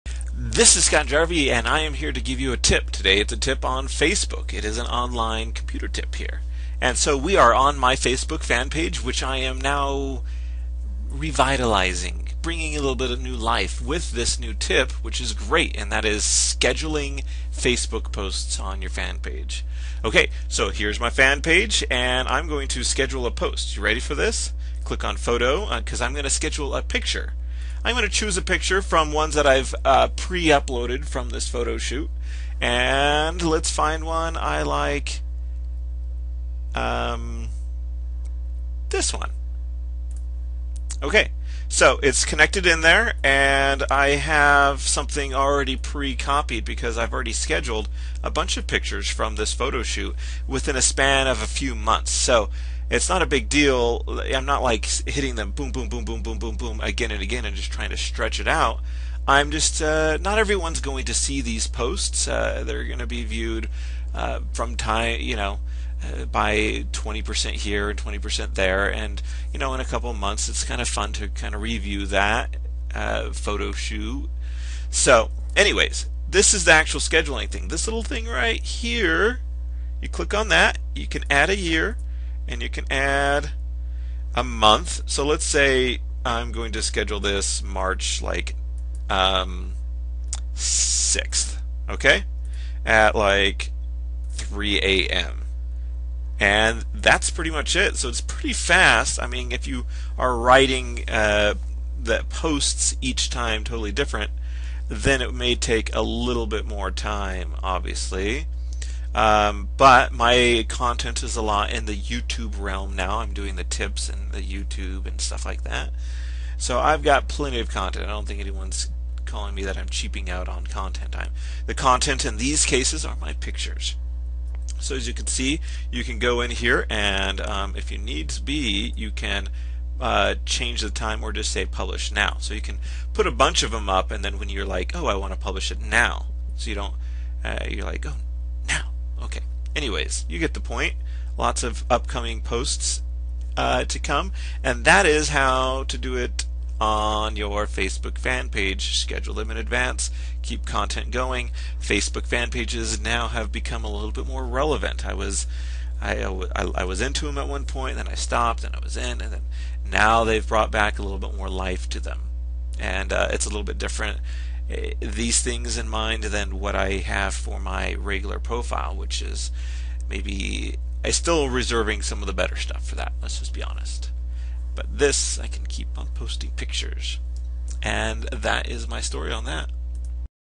Facebook Tip - Scheduling Posts
I'm revitlizing my facebook fan page by pre-scheduling posts.

My content is pictures so I'm able to schedule a ton of picture posts in advance.